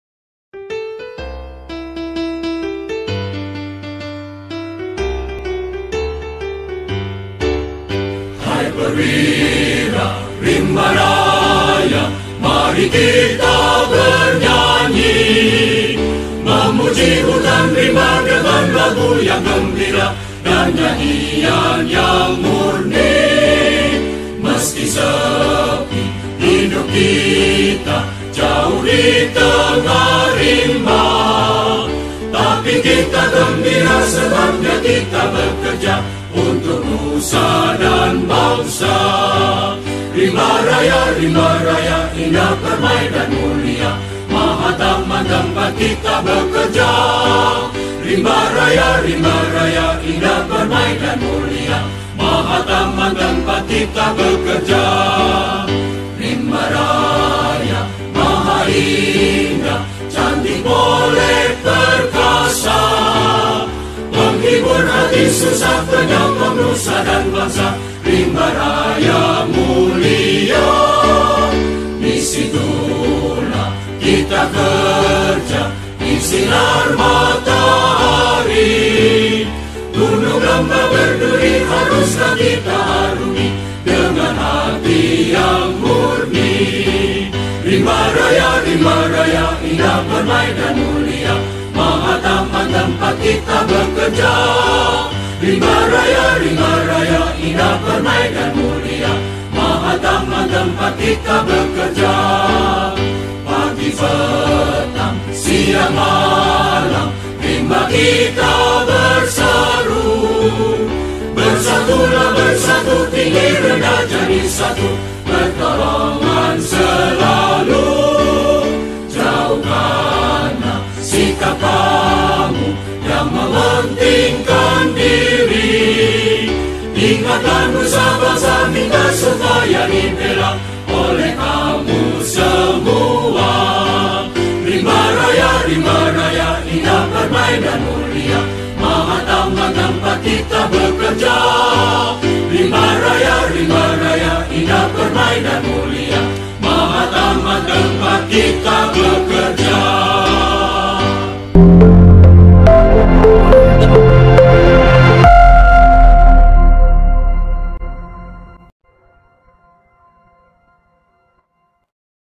Selamat Hari Bakti Rimbawan 2023 Hijaukan Bumi Birukan Langit
Hari Bakti Rimbawan dijadikan sebagai tonggak konsolidasi para rimbawan di seluruh Indonesia untuk kembali menguatkan komitmen dan kesadaran dalam berkarya dan membangun hutan dan kehutanan Indonesia.

Hari Bhakti Rimbawan 2023 diperingati setiap tanggal 16 Maret.